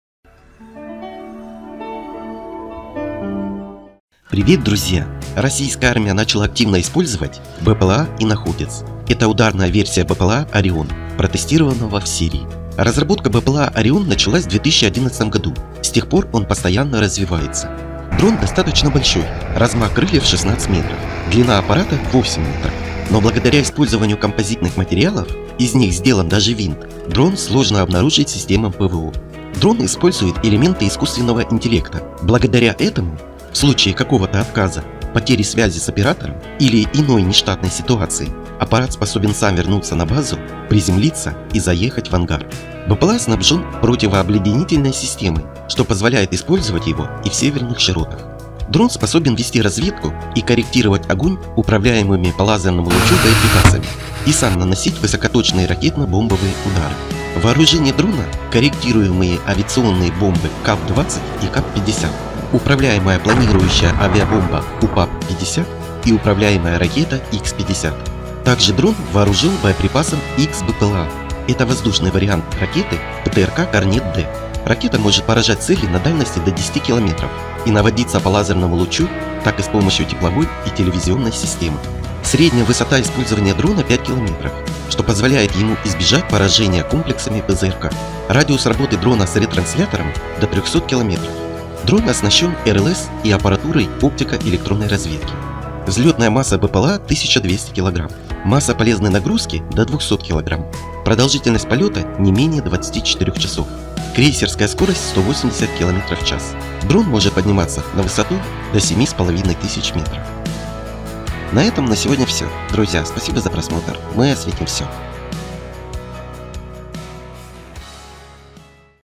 Подробно об ударном дроне Орион России
Российская армия начала активно использовать БПЛА «Иноходец», это ударная версия БПЛА «Орион» протестированного в Сирии. Разработка российского БПЛА «Орион» началась в 2011 году, с тех пор он постоянно развивается. Дрон достаточно большой, размах крыльев - 16 метров, длина аппарата - 8 метров, но благодаря использованию композитных материалов, из них сделан даже винт, дрон сложно обнаружить системам ПВО. Дрон использует элементы искусственного интеллекта, благодаря этому, в случае какого-то отказа, потери связи с оператором или иной нештатной ситуации, аппарат способен сам вернуться на базу, приземлиться и заехать в ангар. БПЛА снабжен противообледенительной системой, что позволяет использовать его и в северных широтах. Дрон «Иноходец» способен вести разведку и корректировать огонь управляемыми по лазерному лучу боеприпасами и сам может наносить высокоточные ракетно-бомбовые удары. Вооружение дрона корректируемые авиационные бомбы КАБ-20 и КАБ-50, управляемая планирующая авиабомба УПАБ-50 и управляемая ракета Х-50. Так же дрон вооружен боеприпасом Х-БПЛА, это воздушный вариант ракеты ПТРК "Корнет-Д", ракета  может поражать цели на дальности до 10 км и наводиться по лазерному лучу и с помощью тепловой и телевизионной системы. Средняя высота использования дрона 5 километров, что позволят ему избежать поражения комплексами ПЗРК. Радиус работы дрона с ретранслятором до 300 км. Дрон оснащен РЛС и аппаратурой оптико-электронной разведки. Взлетная масса БПЛА - 1200 кг, масса полезной нагрузки до 200 кг, продолжительность полета - не менее 24 часов, крейсерская скорость 180 км/час, дрон может подниматься на высоту до 7500 метров.